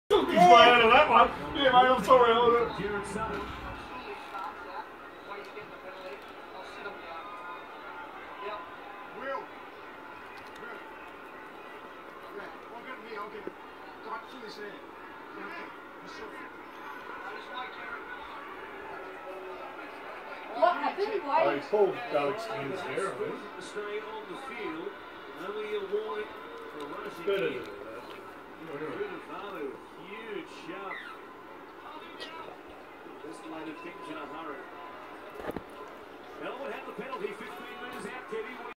Roxy having pizza while watching NRL.
Dog, irish wolfhound cross.